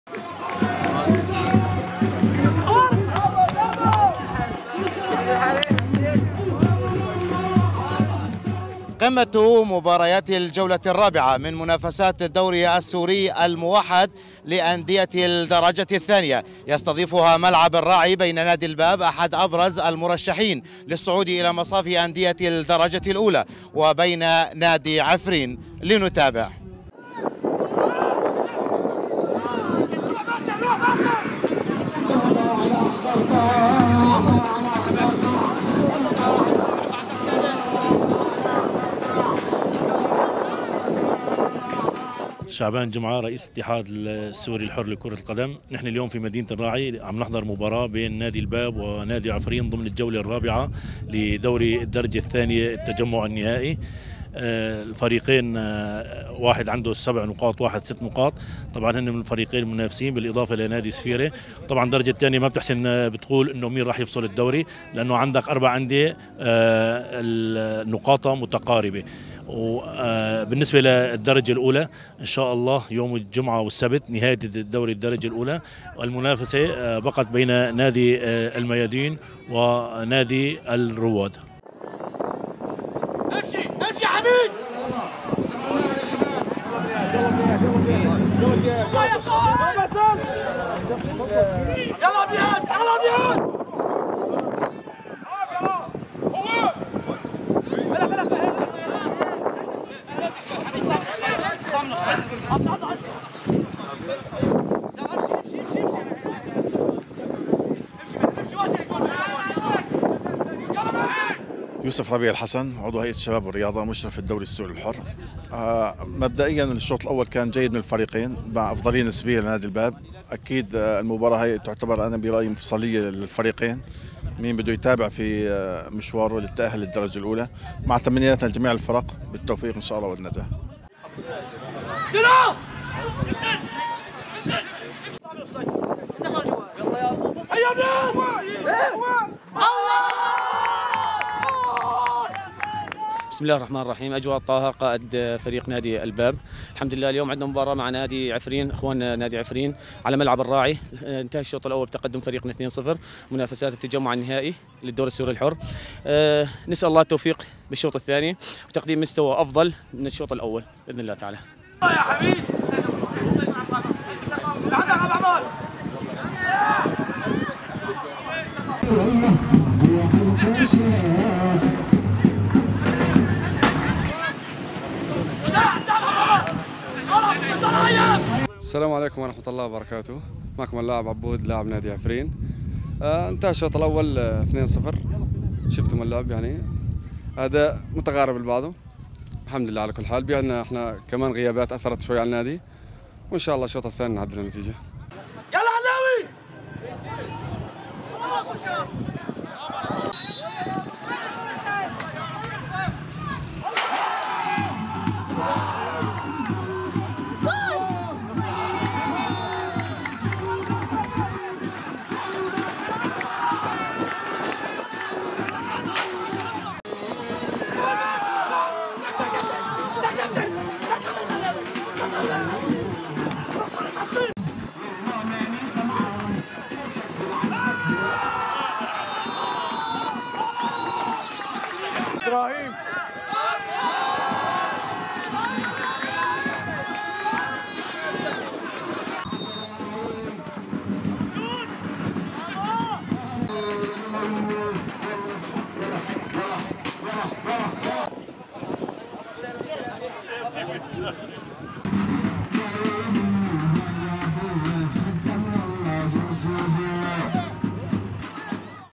نادي الباب يتفوق على نظيره نادي عفرين بنتيجة 4-0 في اللقاء الذي جمع الفريقين على أرضية ملعب الراعي
في قمة مباريات الجولة الرابعة من منافسات الدوري السوري الموحد لأندية الدرجة الثانية، تفوق نادي الباب على نظيره نادي عفرين بنتيجة 4-0، في اللقاء الذي جمع الفريقين على أرضية ملعب الراعي.